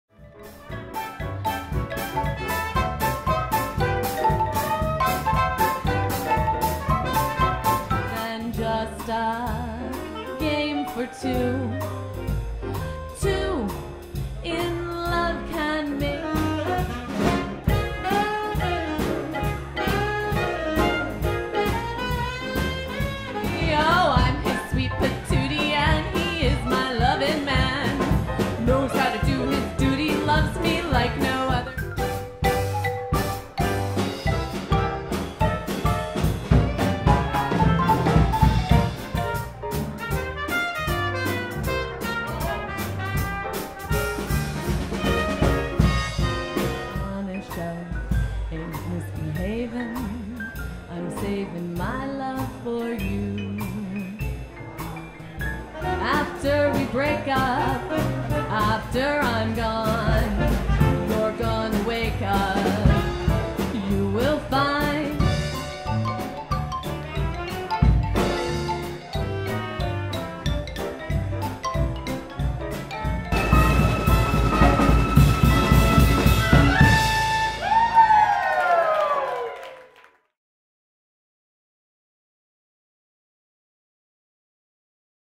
Roaring 20s Gatsby Band Orlando, FL
Authentic 1920s themed band based in Orlando, Florida, featuring Heather Thorn- xylophone and vocals with:

Eddie  Metz Jr- drums
Dave Mackenzie- reeds
John Depaola- trumpet
Greg Zabel- upright bass
Jeff Philips- piano

Perfect for private parties, community and corporate events, swing dances, themed weddings, retirement communities, concerts and more!